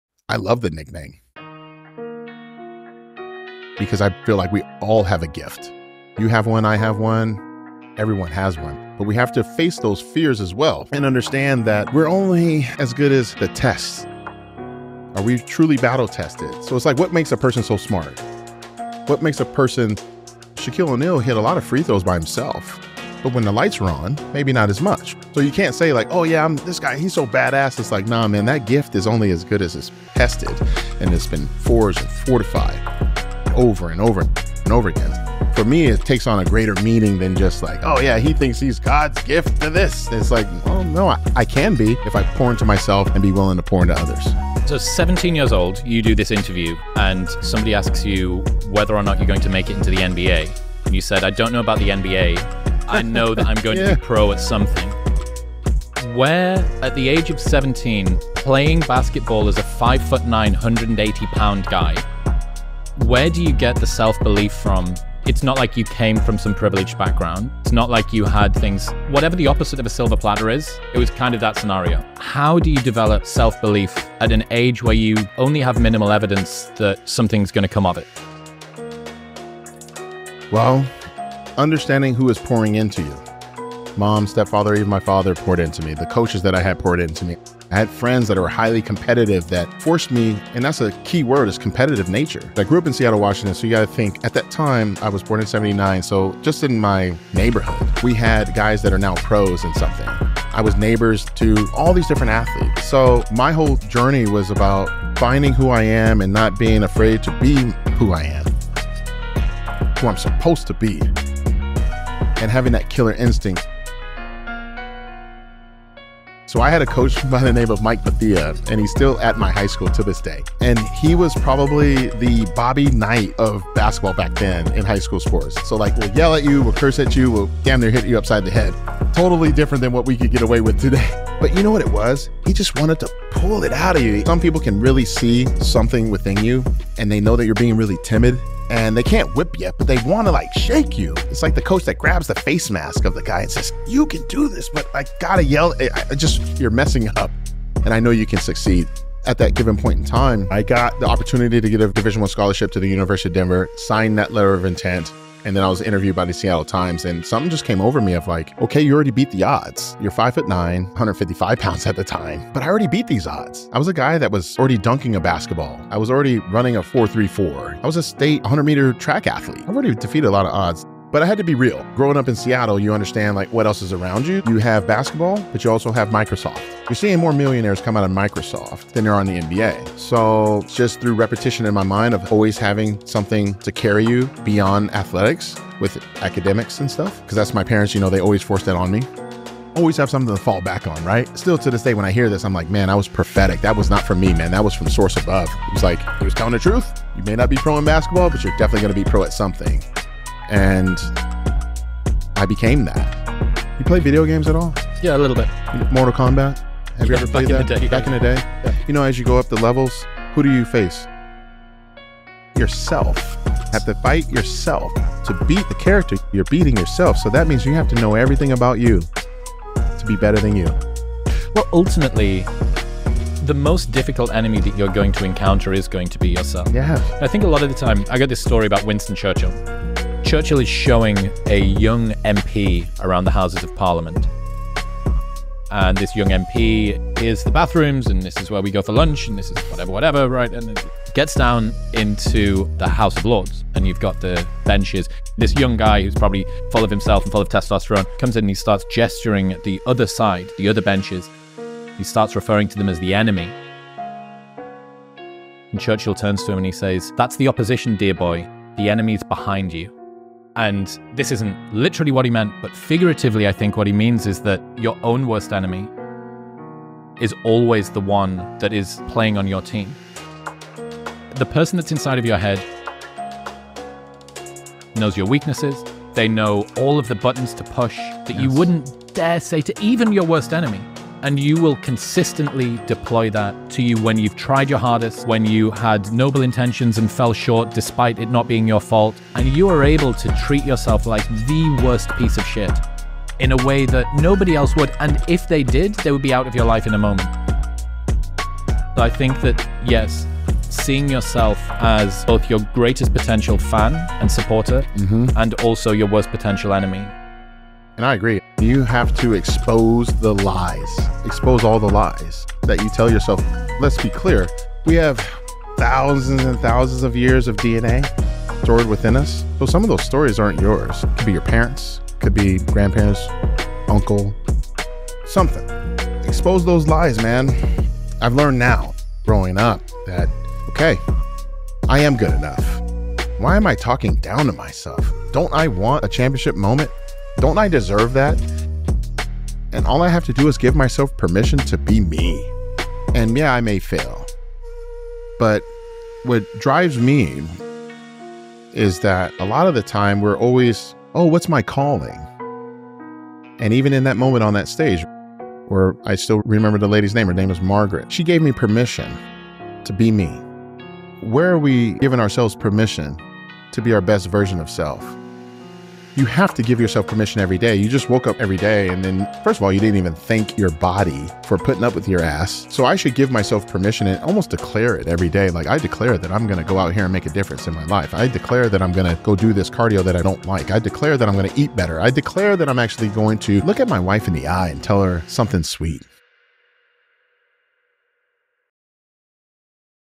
Phil Heath Chris Williamson Give Yourself Permission To Be Your Best Self Inspirational Music Video
Phil Heath & Chris Williamson 🌟 Give Yourself Permission To Be Your Best Self ✨🌱
An Inspirational Music Video 💫 🎶  📹
Instrumental Music by Gravy Beats 🥁🔊
Arranged and Edited by BeatVox 2023 🗣🥁💫
You Are What You Listen To! ✨
Listen to good stuff! 🌱 💥 🌟
BeatVox - More & Elswhere: